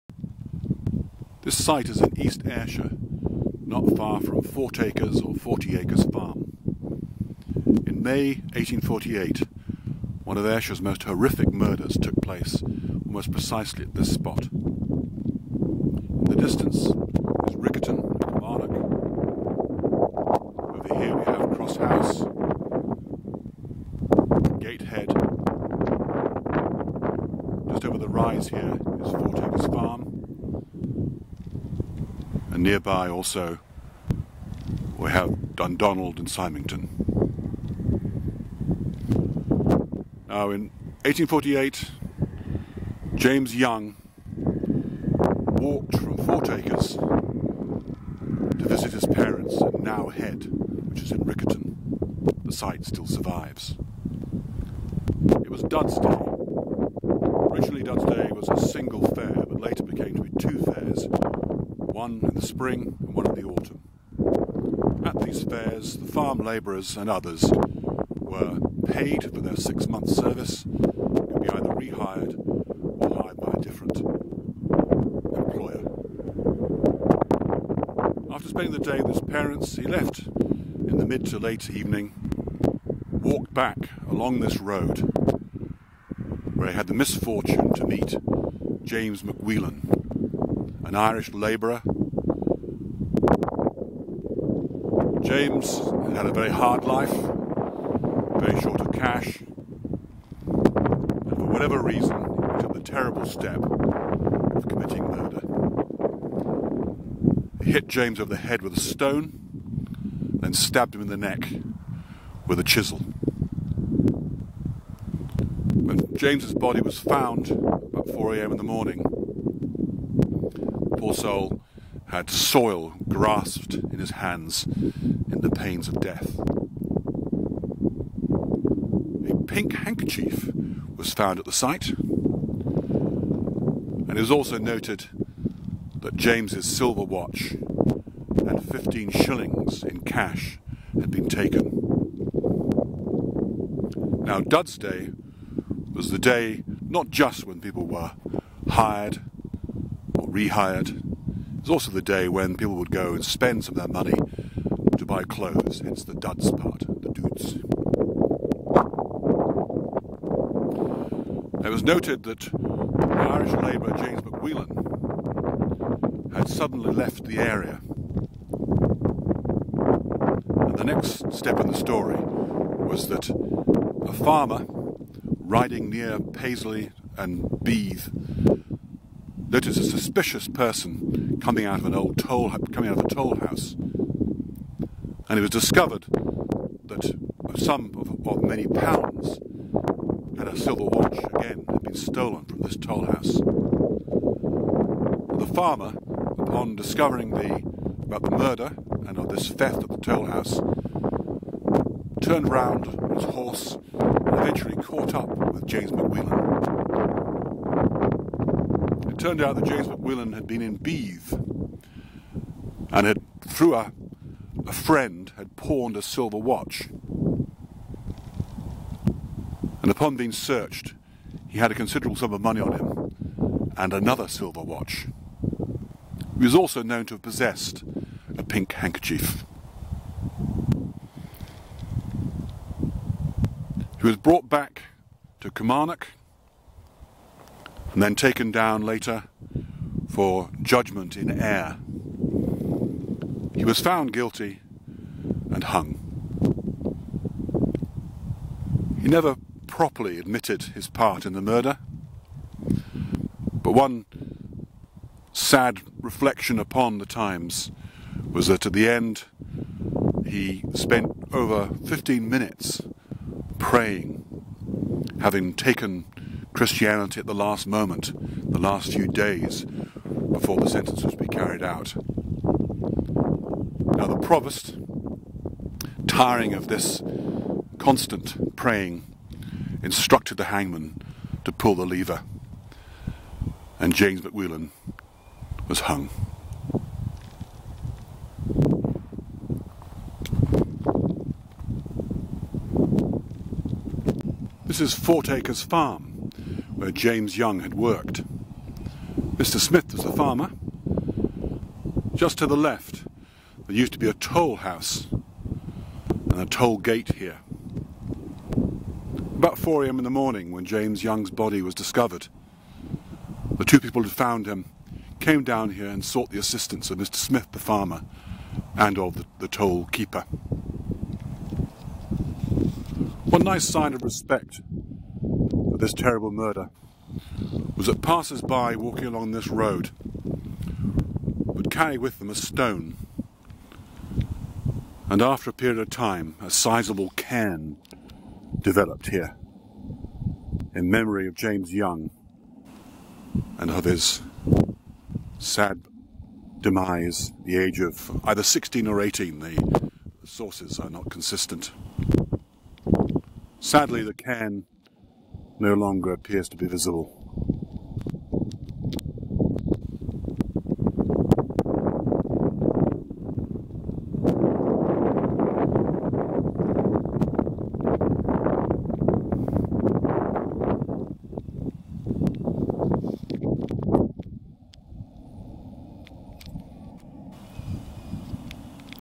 The Murder of James Young in 1848, Symington, Ayrshire
Said to be one of Ayrshire's most cruel murders. James Young was only 18 and on hos way back from the fair when he was knocked down, stabbed in the neck, robbed and left to bleed to death.